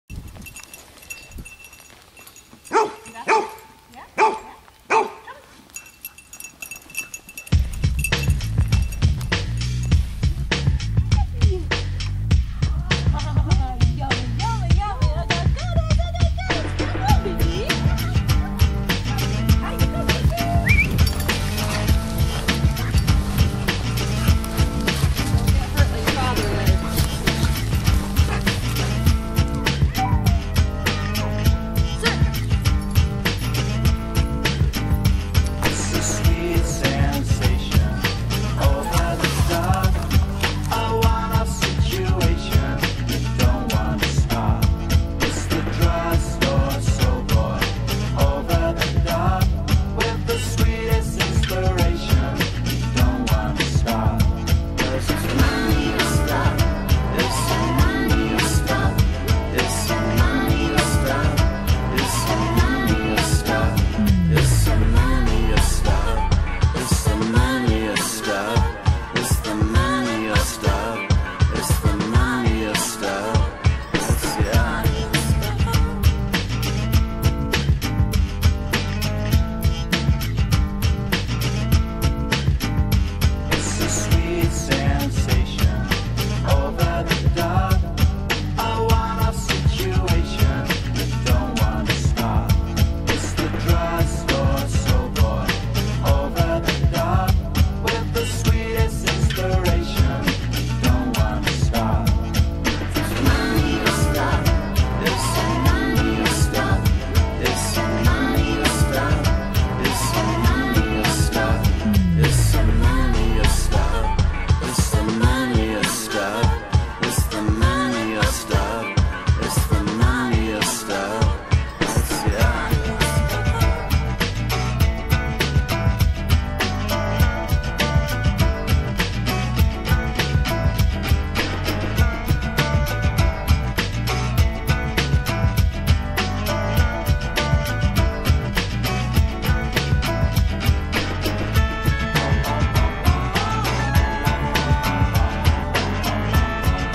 SAR Practice with Mika
Mika (King shepherd pup)-Training to be a SAR dog
Emmy(German Shepherd pup)-Training to be a SAR dog
Amy(German Shepherd)-Almost operational
Libby(golden Retriever)Operational SAR dog
Angel(Pitbull)-Pet dog
Hanging out and working at SAR practice in PA at a Quarry. This is the first time Emmy and Mika have been in a quarry so this was very exciting for them.